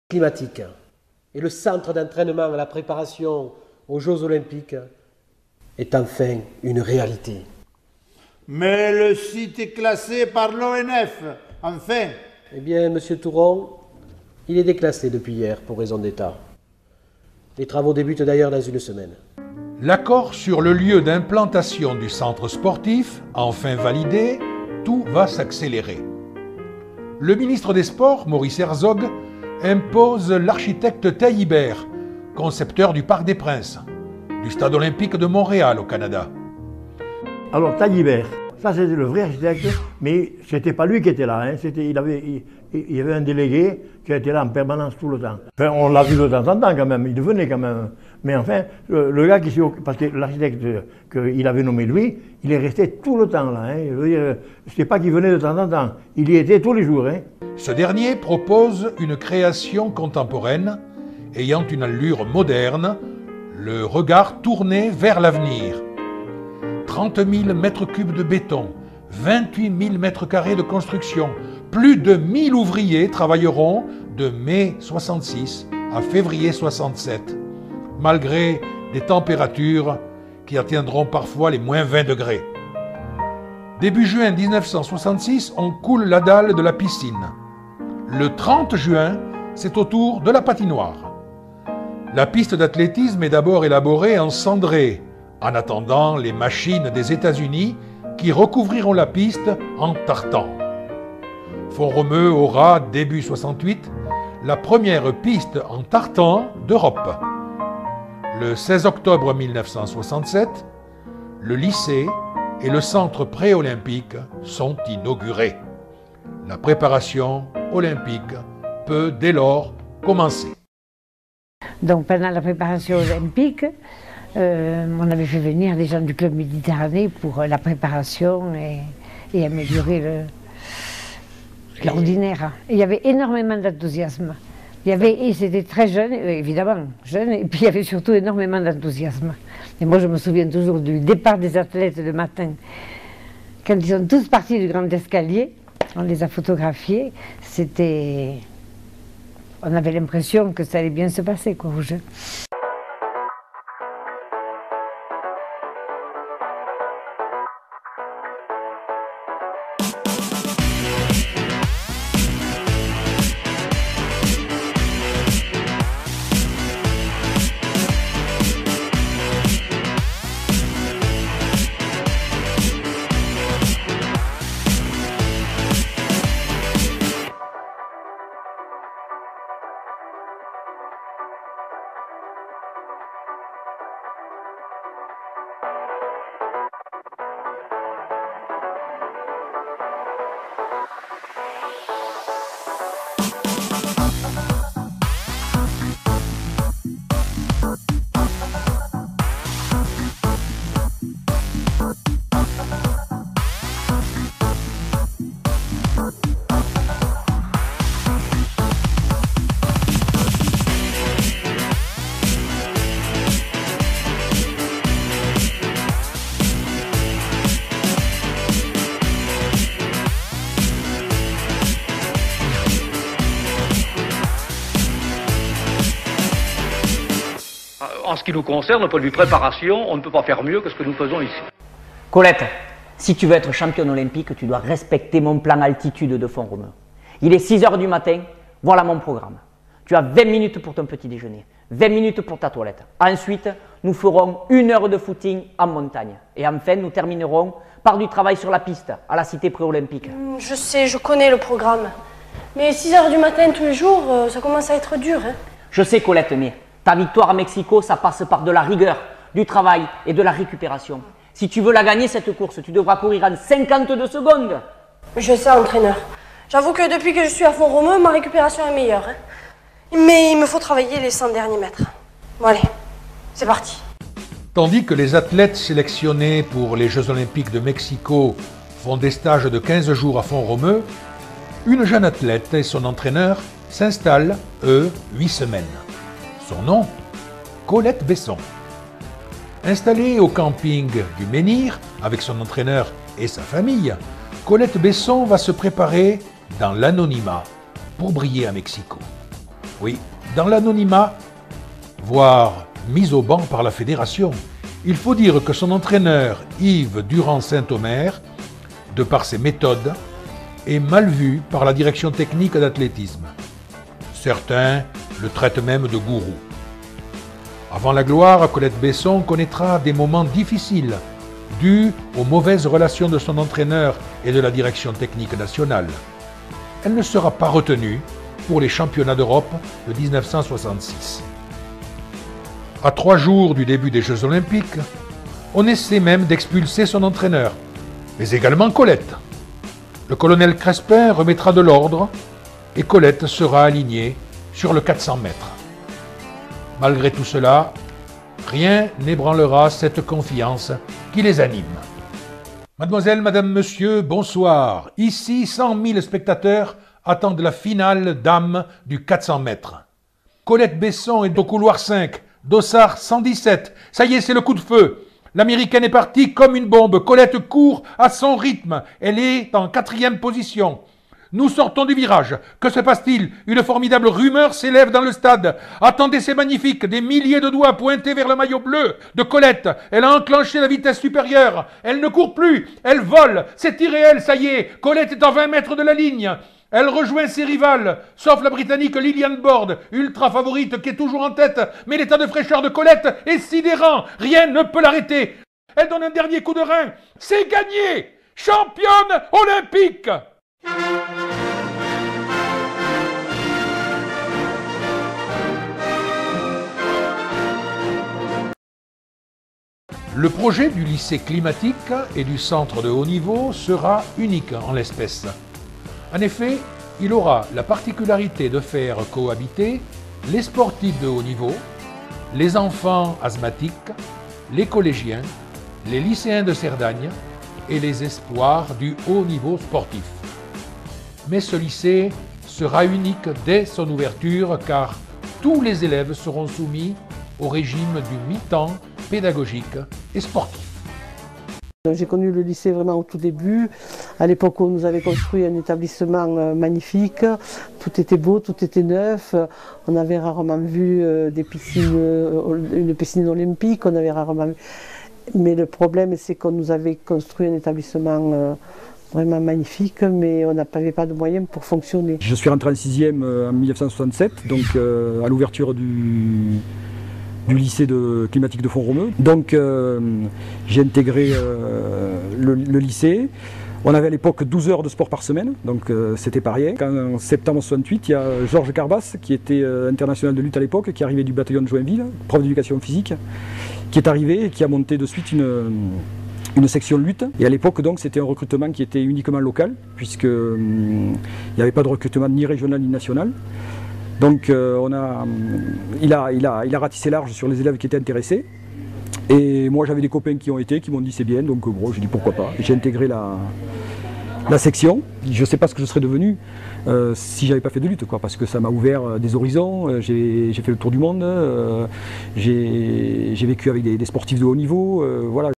l'Histoire de Font-Romeu 3ème partie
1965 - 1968
Maurice Herzog, Colette Besson... JO de Mexico
Jerez, Pic, Marti...
Lycée Climatique et Sportif, Centre pré-olympique, 1ère piste en tartan d'Europe, piscine, centre équestre, escrime, les Bouillouses